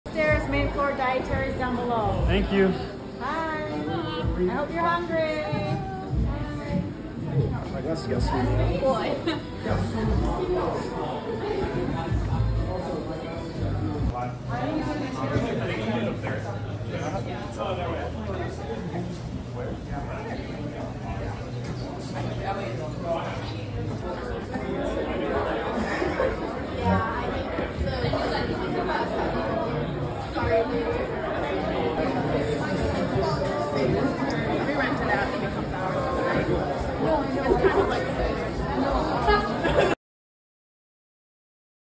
U-M Marching Band Thanksgiving Tradition
The University of Michigan Marching Band honors their annual Thanksgiving tradition by eating dinner at Cottage Inn the day before the Ohio State game in Ann Arbor, Michigan, on Friday, November 24, 2023.